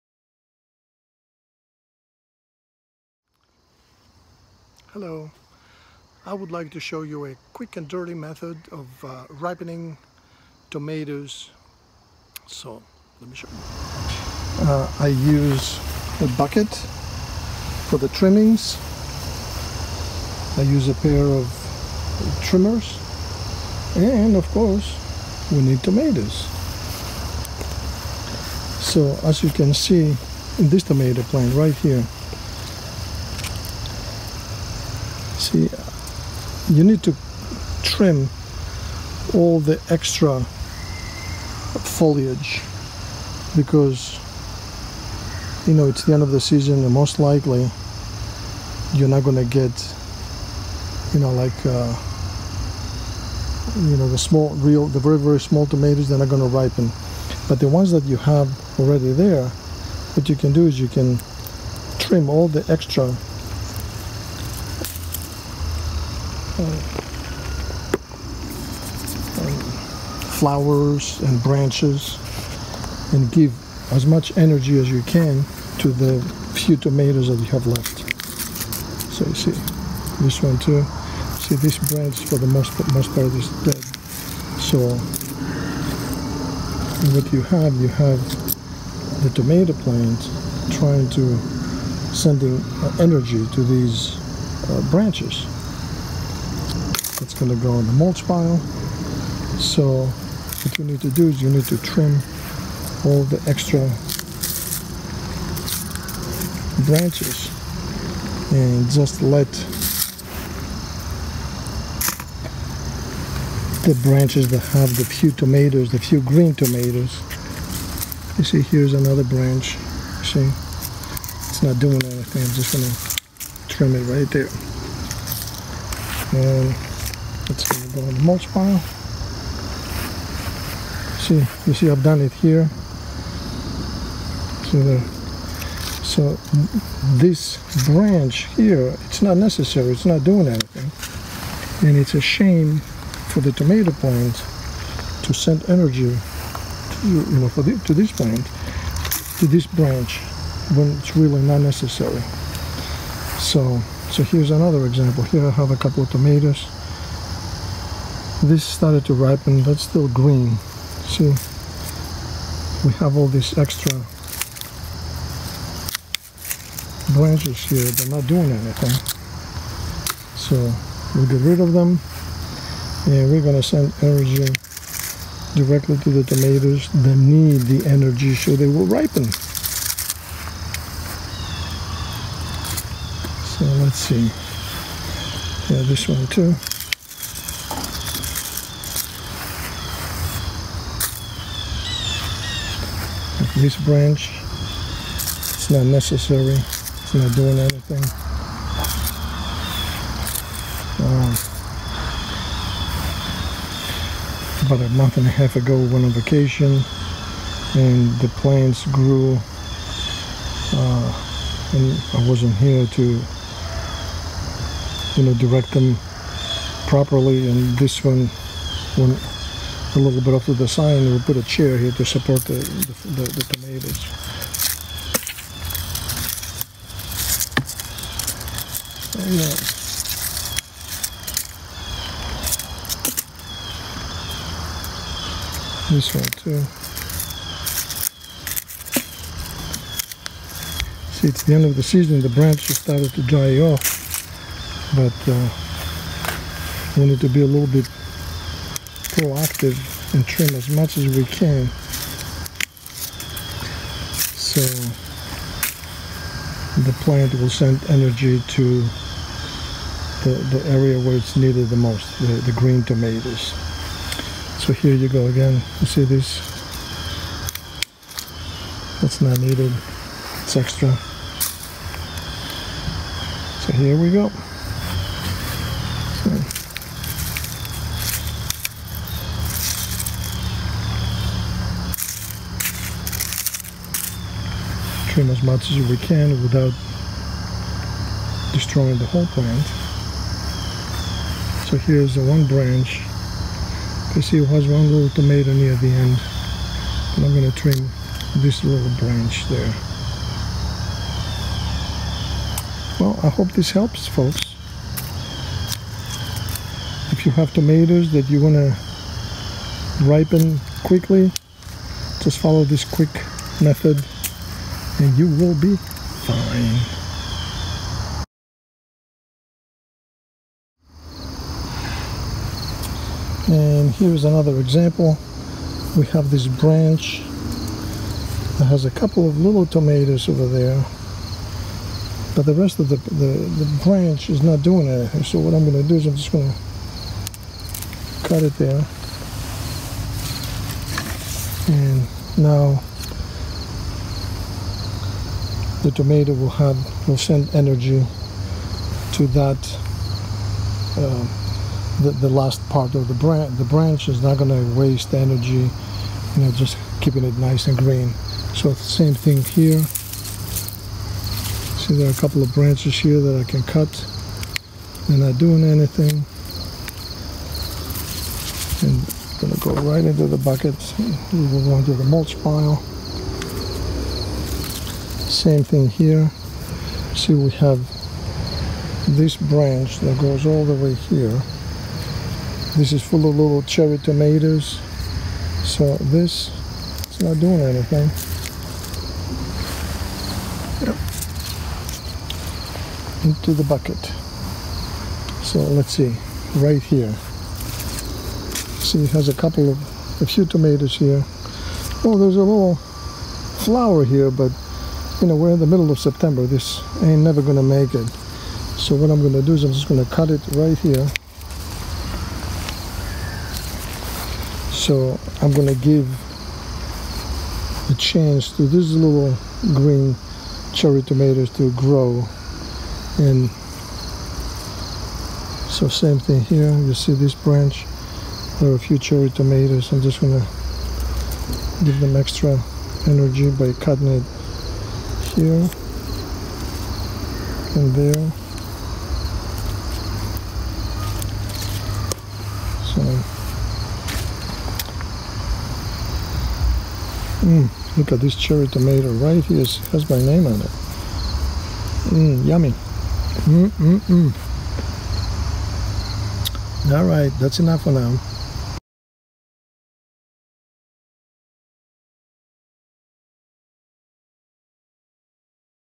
How to Force-Ripen Green Tomatoes
The tomato vines are full of green tomatoes and I need to do something to help them ripen QUICKLY!

Trimming any extra branches will help to ripen green tomatoes because the plant will not waste sending energy to areas where there are no tomatoes.